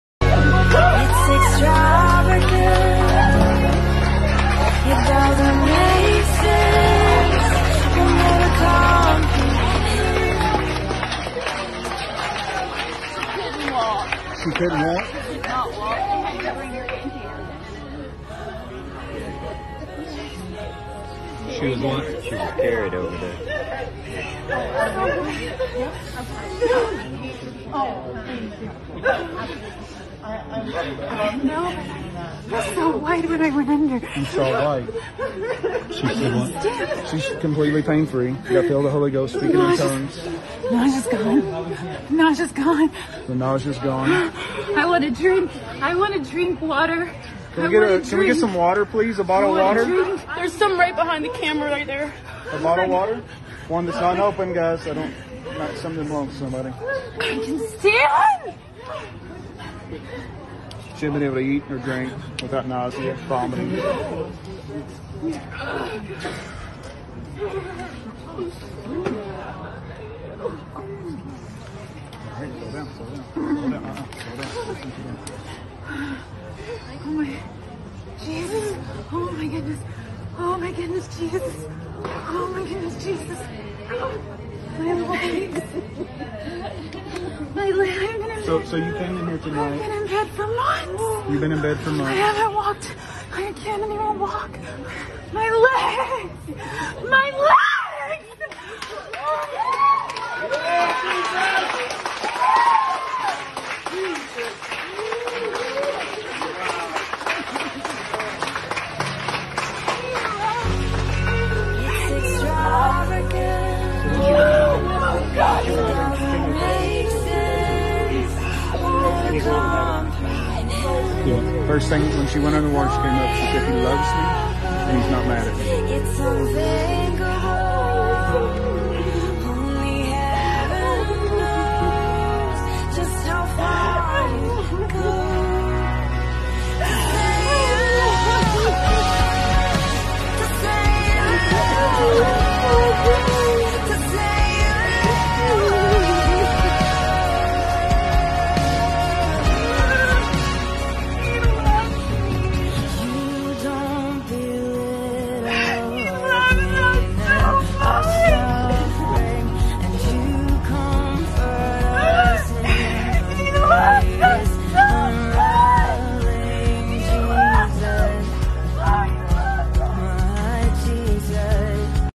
A Bedridden Woman Gets Healed During Baptism by Jesus
A Bedridden Woman Gets Healed During Baptism by Jesus & the substitles are in Portuguese. Miracles still happen today they've supernatural so is healing. "Greater things than these shall you do because I go to my Father." - Yeshua Messiah Jesus Christ. I am not sure where this happened or who it happened to all I know is that this woman was healed so because Jesus is no respecter of persons this same healing is available to you.